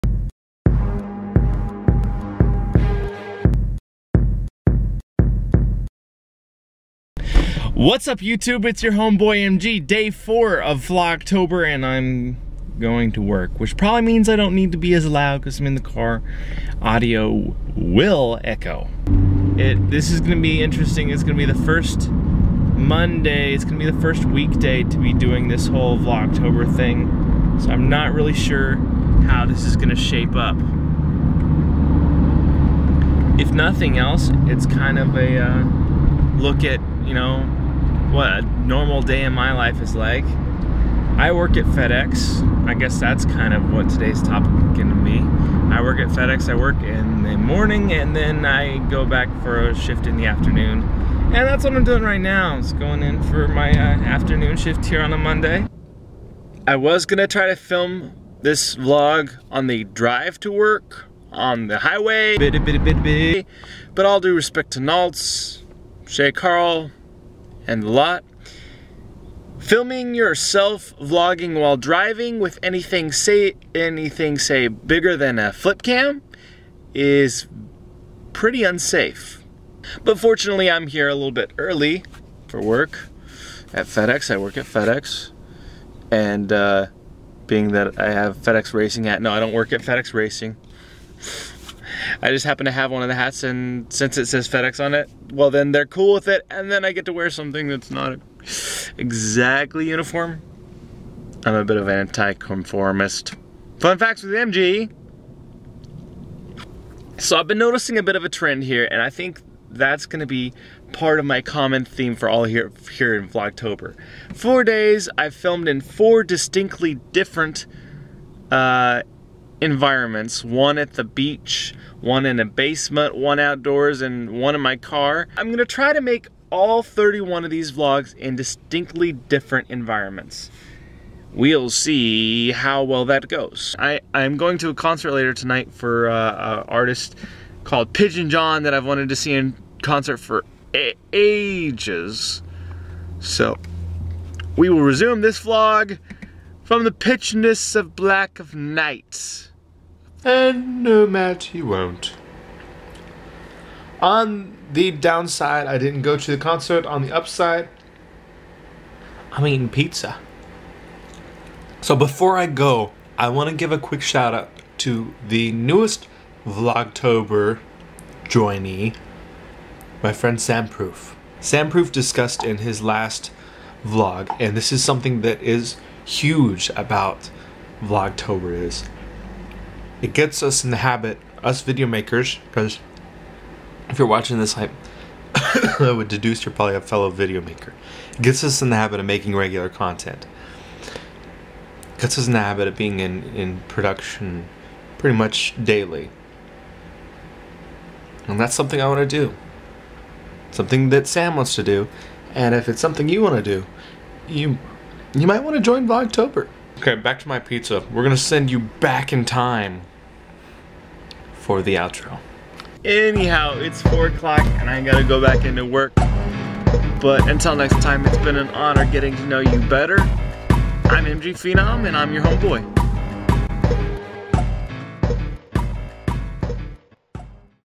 VLOGTOBER: Ten Four Good Buddy! 10.04
MG drives to work and talks about his work... and his job... and plans for the remainder of vlogtober! Tomorrow should be more exciting :P what did you guys do today?

Special welcome and a shoutout to new Vlogtober-er and my good friend Sam Proof